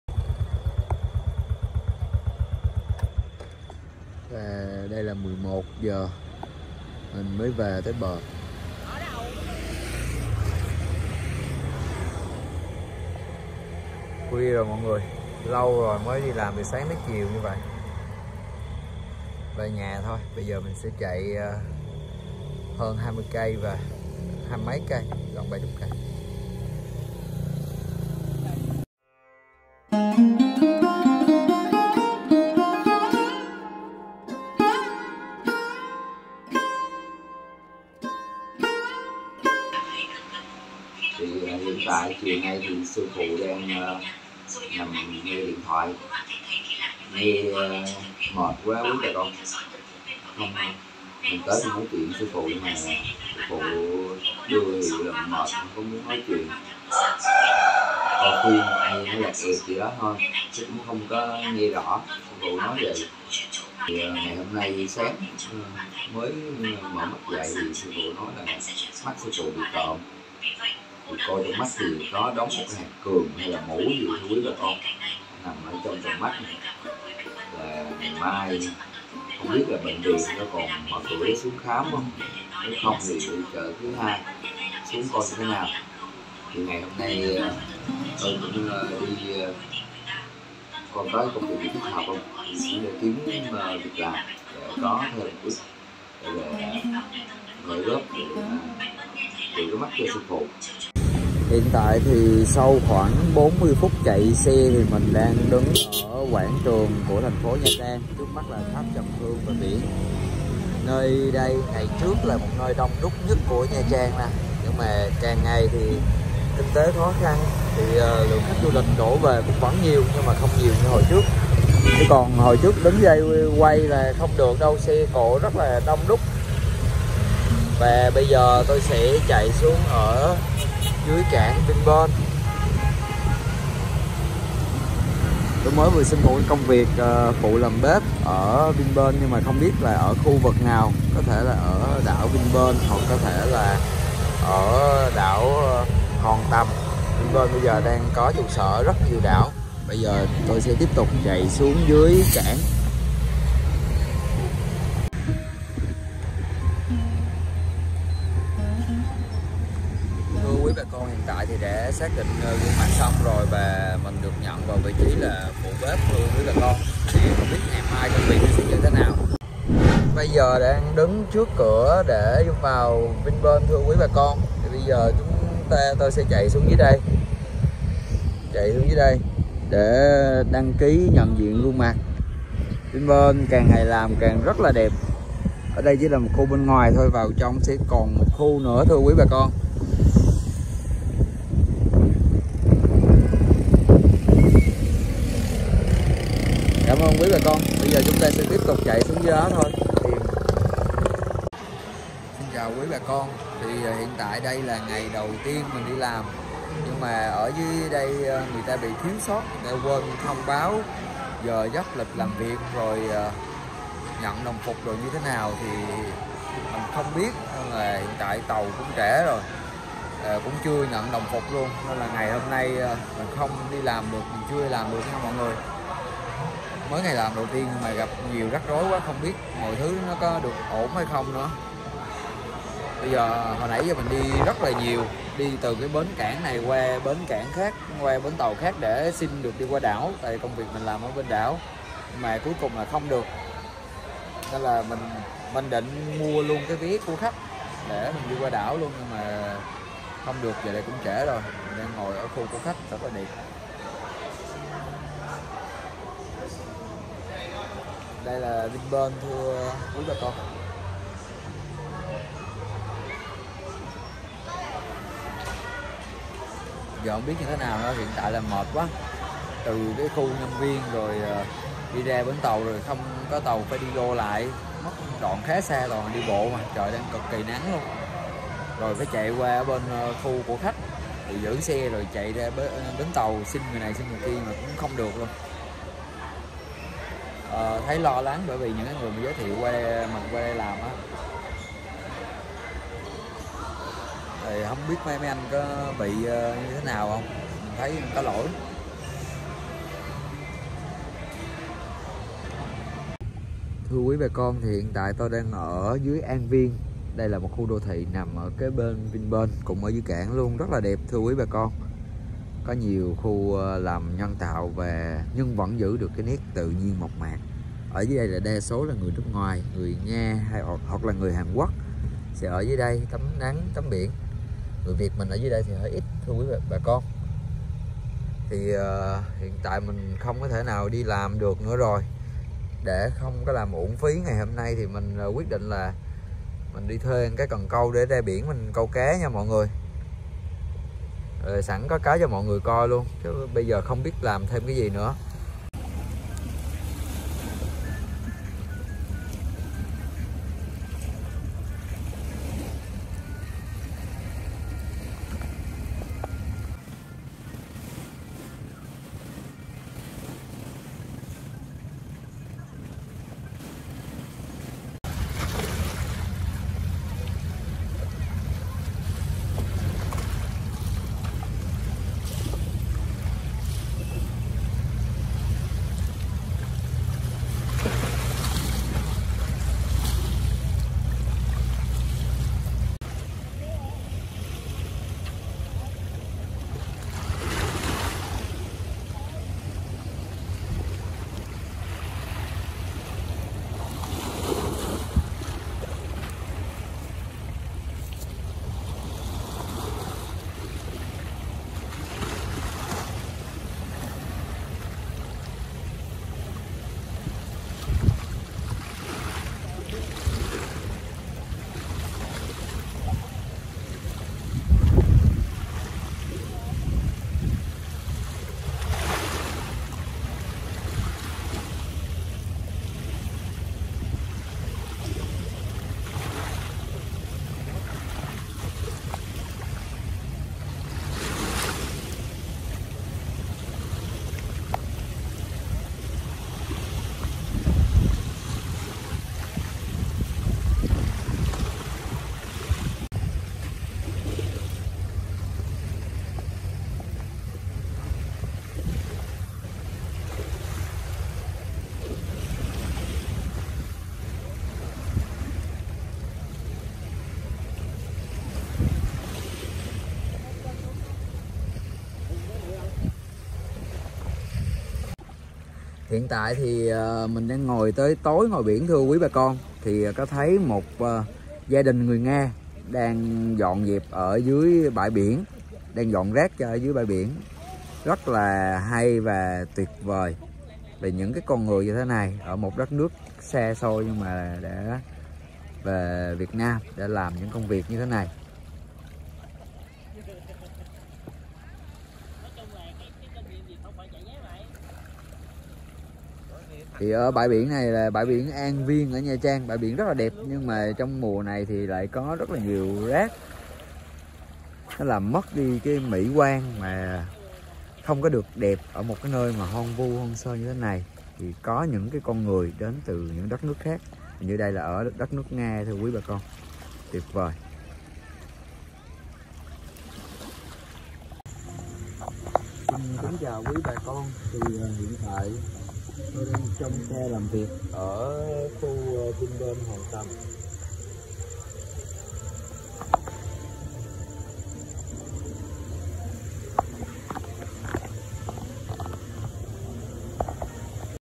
Những ngày không bình thường khi về ở cùng người sư phụ điếc
Những ngày không bình thường khi về ở cùng người su phụ điếc